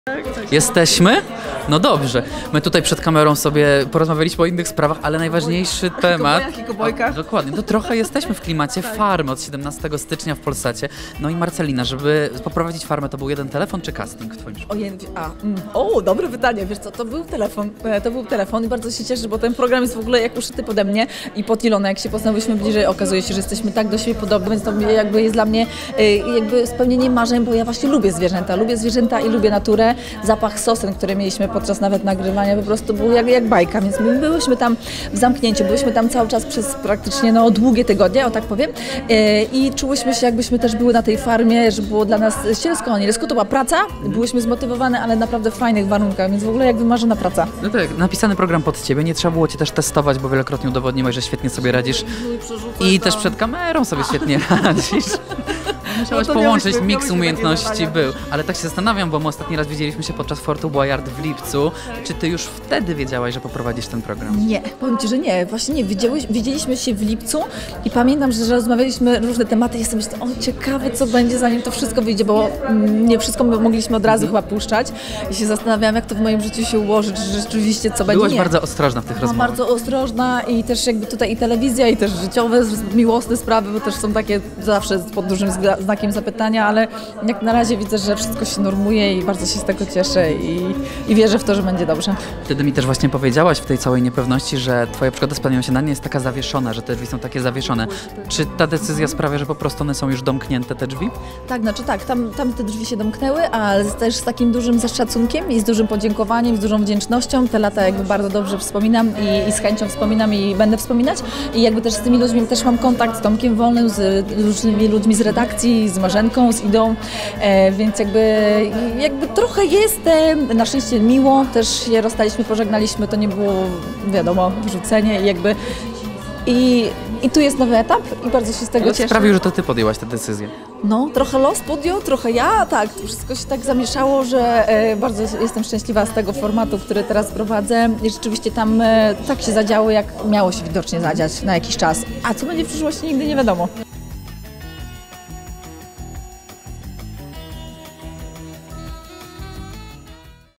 Tak Marcelina Zawadzka rozstała się z TVP: „To nie było wyrzucenie"
W sierpniu 2020 roku Marcelina Zawadzka przestała nagle prowadzić Pytanie na śniadanie. Teraz pracuje w innej telewizji przy show Farma. „Trochę tę decyzję podjął los, trochę ja. Tak się wszystko zamieszało i teraz jestem szczęśliwa z formatu, który prowadzę w Polsacie", mówiła Rafałowi Kowalskiemu.